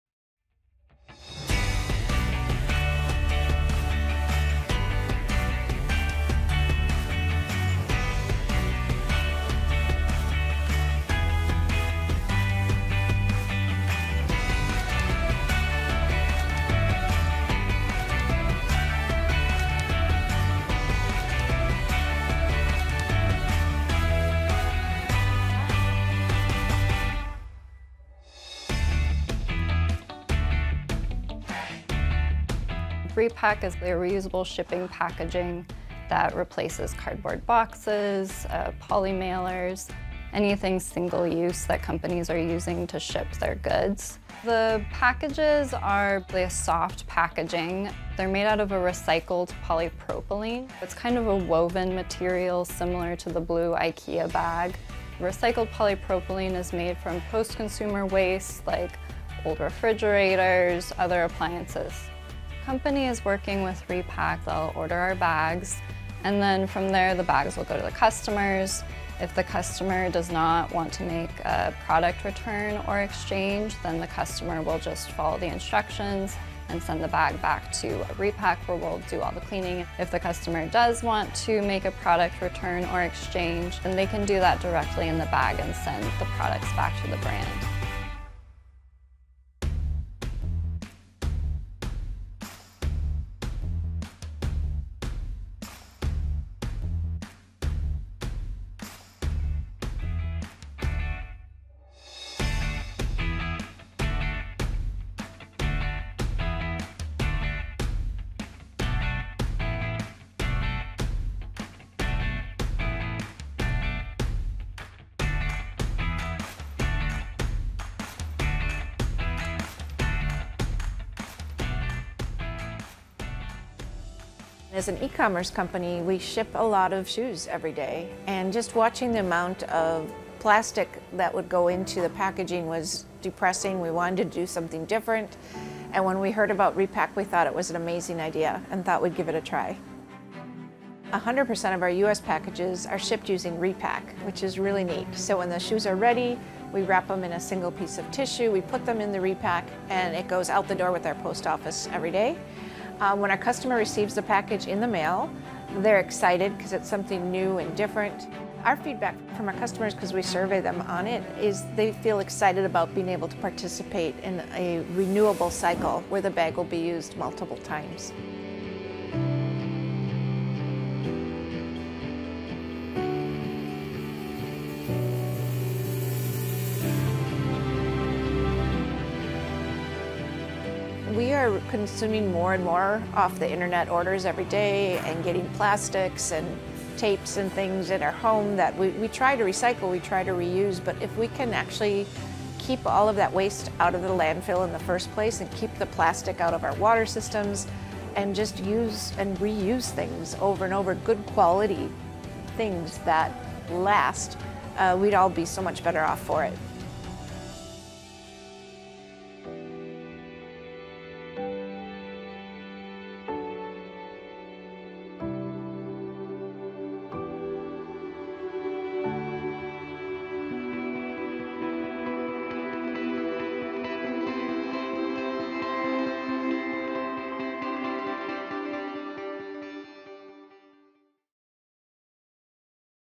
The reusable shipping bags that could solve online shopping's plastic problem | Solving Plastic
In 2018, a total of 13.5 billion packages were shipped in the United States — that's a whole lot of single-use plastic packaging. Companies like RePack are beginning to offer an innovative new business model, leasing reusable bags to retailers that sell goods online. Using a service like RePack can save up to 80% of CO2 emissions from shipping and reduce packaging waste by 99%.

Is there a plastic solution in your community that you'd like us to profile?

This video features RePack, a company operating in Europe and the US.